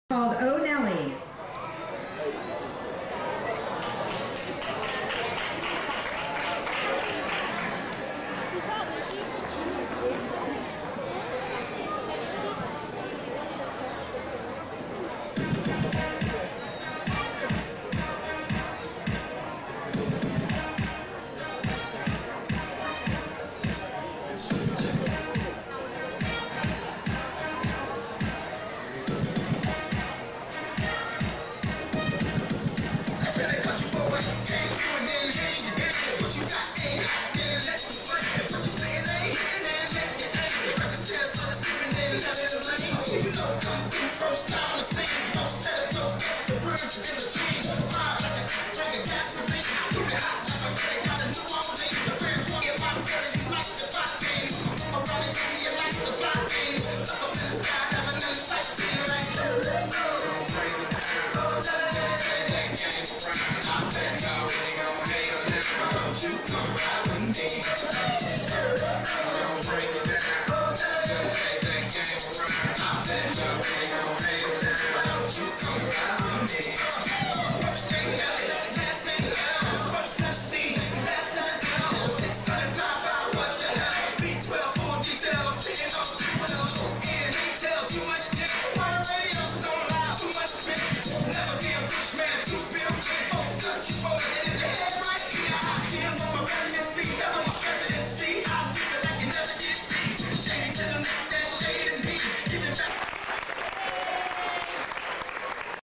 Oh Nelly, Hiphop Jean's Dance Studio
From the 2003 show at the fair. If anyone from this dance studio wants this video removed just ask.